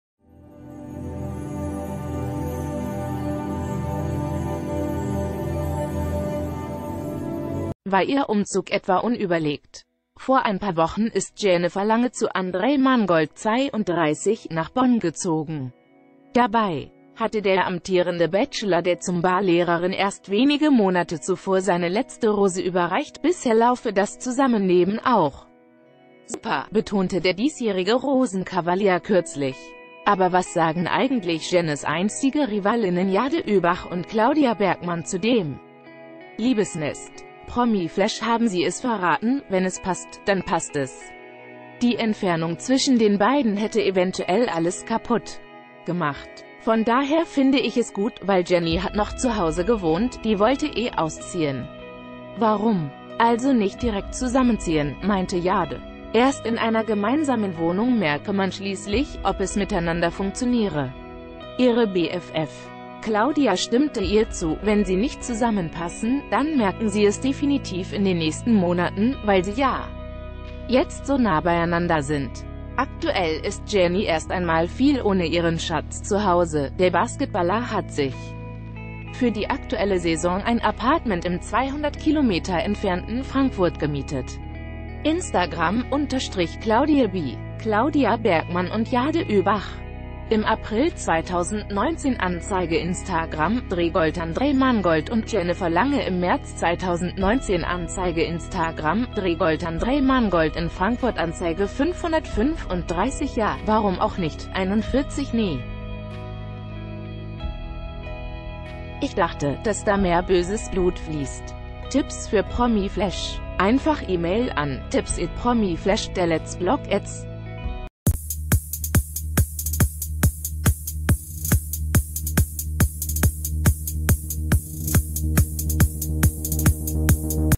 Jenny & Andrejs Umzug übereilt? Das sagen die Bachelor-Girls
War ihr Umzug etwa unüberlegt? Vor ein paar Wochen ist Jennifer Lange zu Andrej Mangold (32) nach Bonn gezogen. Dabei hatte der amtierende Bachelor der Zumba-Lehrerin erst wenige Monate zuvor seine letzte Rose überreicht – bisher laufe das Zusammenleben auch super, betonte der diesjährige Rosenkaval...